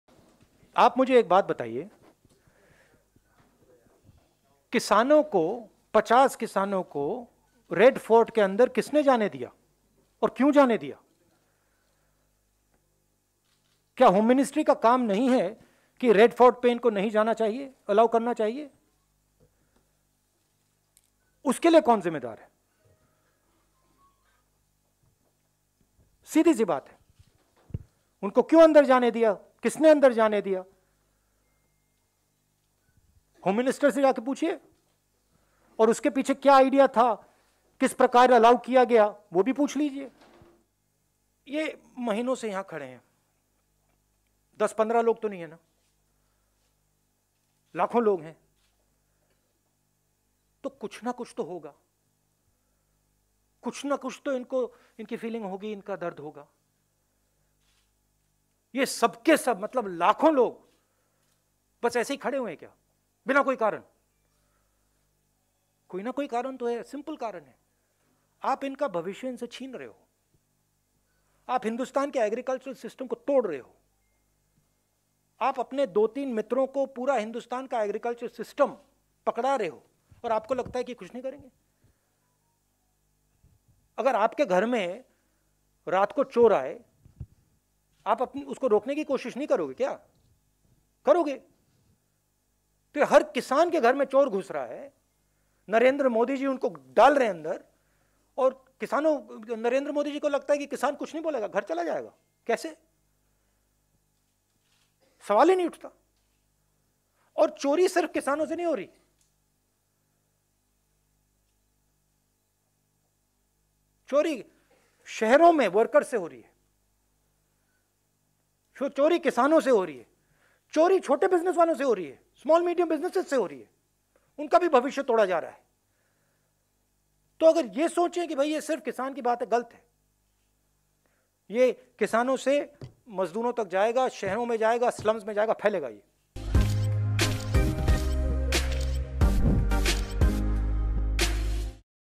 Tractor Parade Violence: Rahul Gandhi ने Modi Government से पूछा ये सवाल | वनइंडिया हिंदी
Former Congress president Rahul Gandhi held a press conference on the fierce demonstration against agricultural laws. Rahul Gandhi put a question mark on the demonstration on the Red Fort on Republic Day. He asked why people were allowed to enter the Red Fort. Why were they not stopped? Ask the Home Minister what the purpose was, to allow those people inside the premises.

कृषि कानूनों के खिलाफ हो रहे उग्र प्रदर्शन पर कांग्रेस के पूर्व अध्यक्ष राहुल गांधी ने प्रेस कॉन्फ्रेंस की.  गणतंत्र दिवस के दिन लाल किले पर हुए प्रदर्शन पर राहुल गांधी ने प्रश्न चिह्न लगया. उन्होंने पूछा कि लाल किले में लोगों को जाने की अनुमति क्यों दी गई? उन्हें रोका क्यों नहीं गया? गृह मंत्री से पूछें कि उद्देश्य क्या था, उन लोगों को परिसर के अंदर जाने देना.